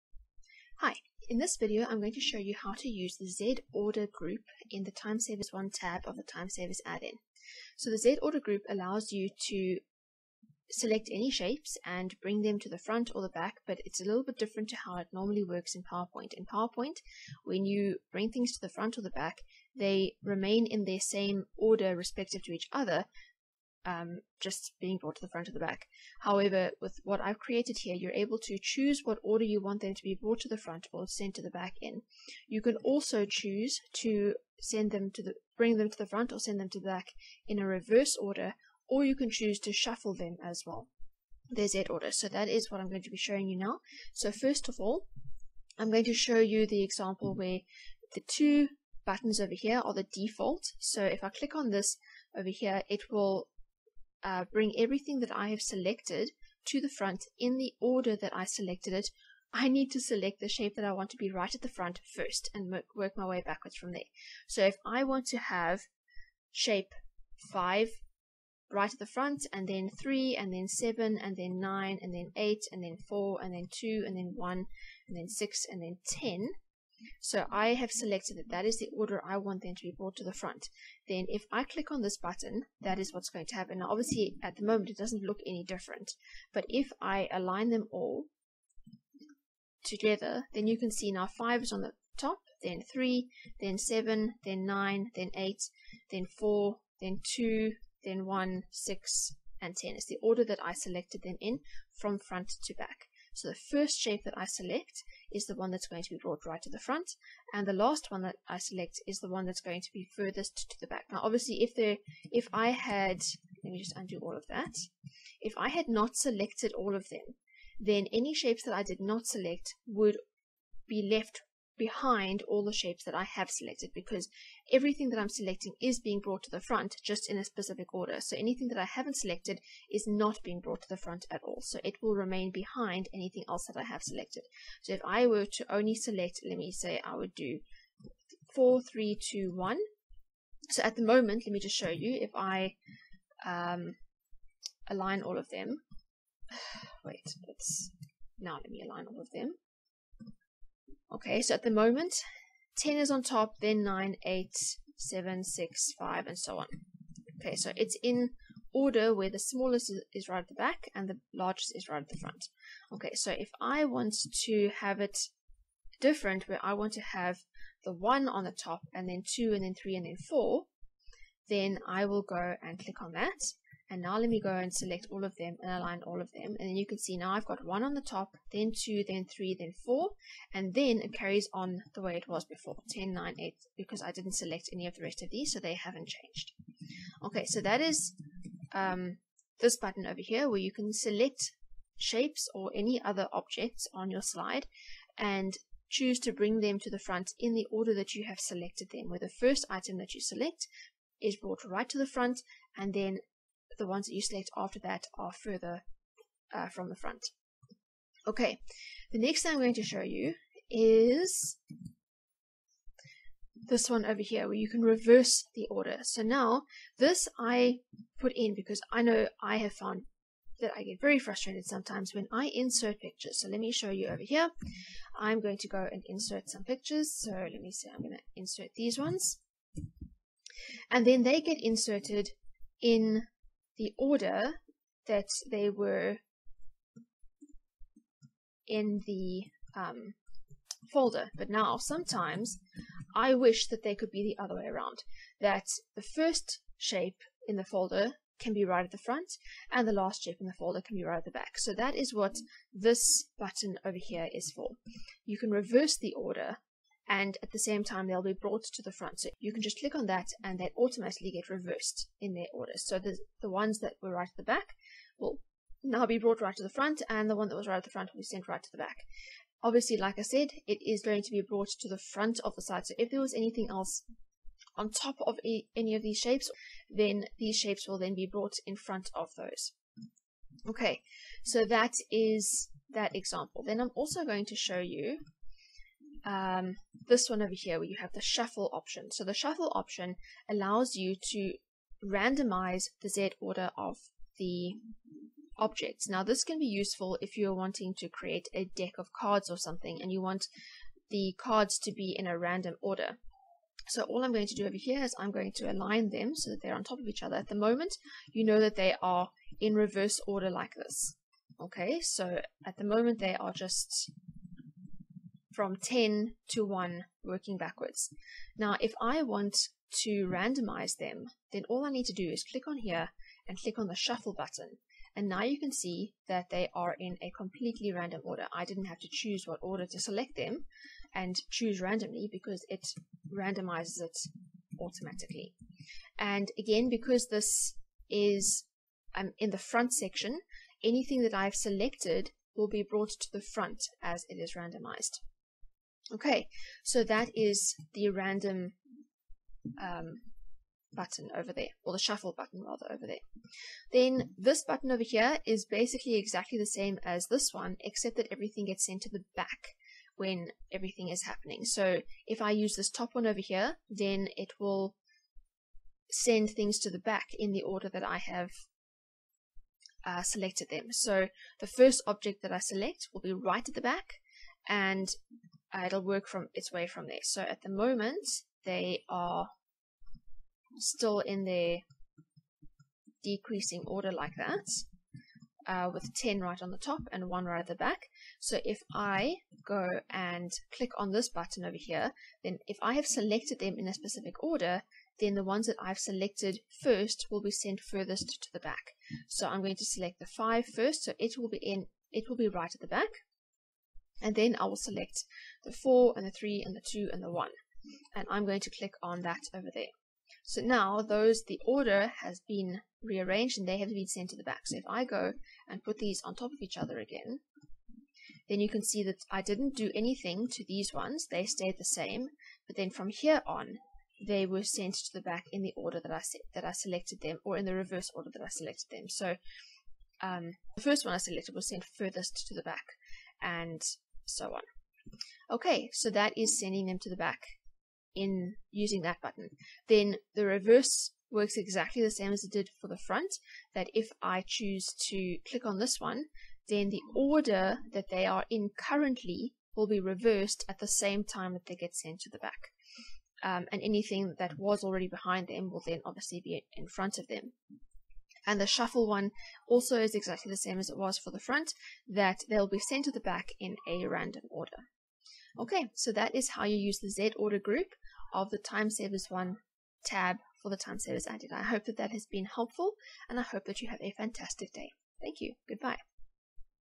How to use the Z-Order group in the Time Savers Add-in
In this video I show you how to use the Z-Order group in the Time Savers Add-in for PowerPoint from Colleen's Cosmic Collection.